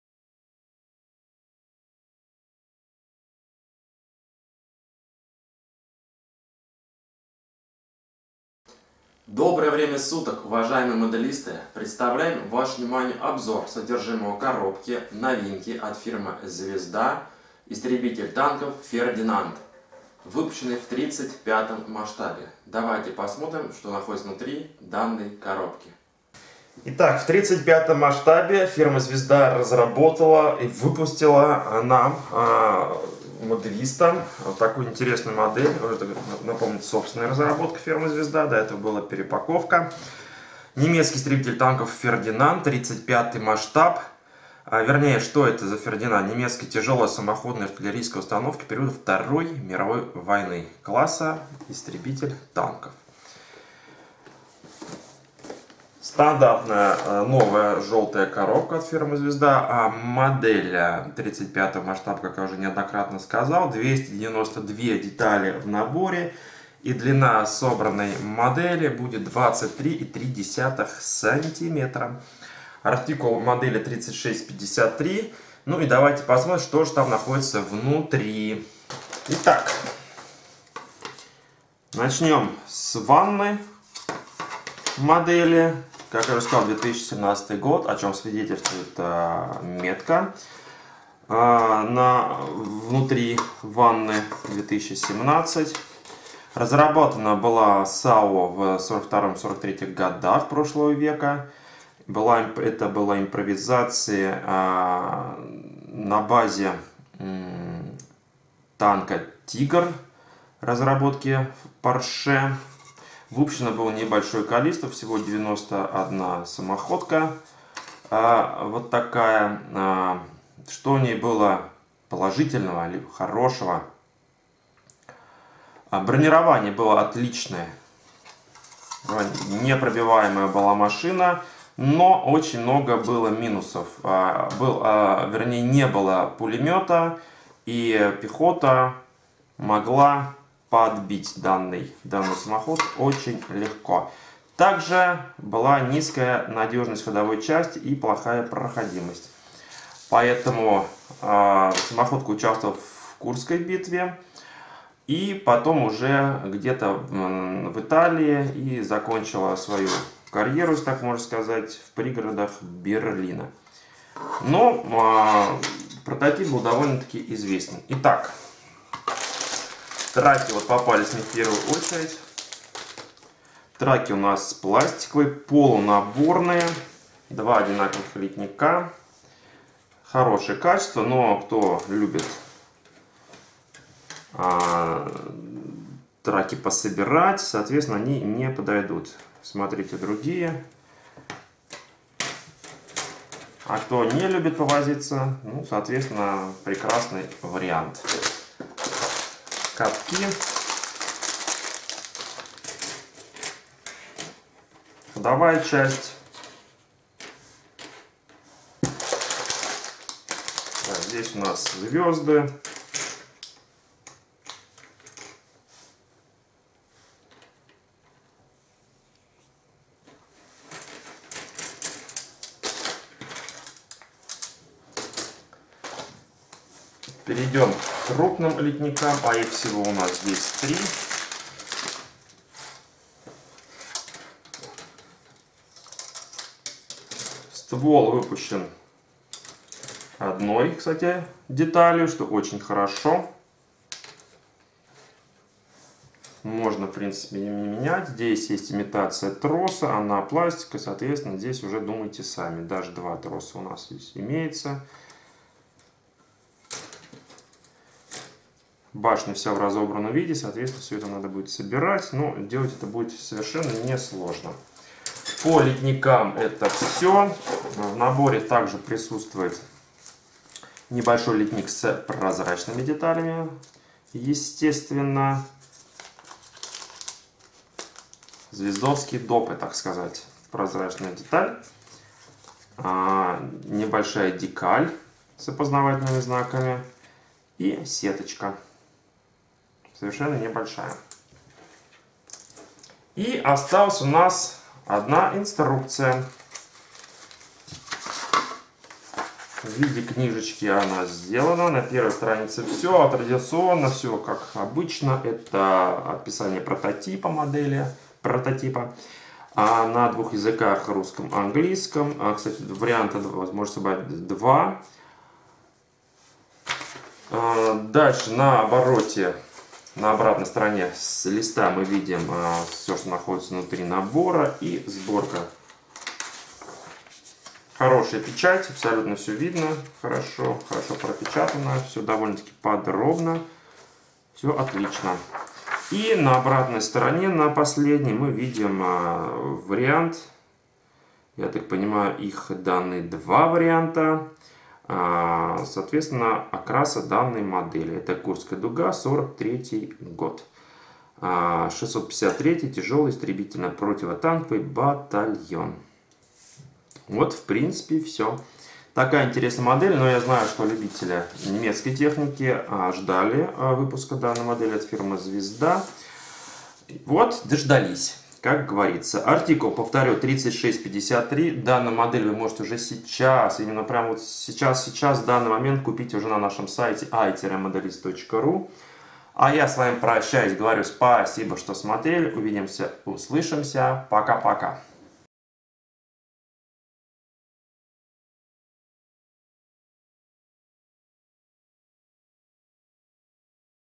Обзор модели Фердинанд
Обзор содержимого коробки сборной масштабной модели фирмы "Звезда": немецкий истребитель танков "Фердинанд", выполненной в масштабе 1/35.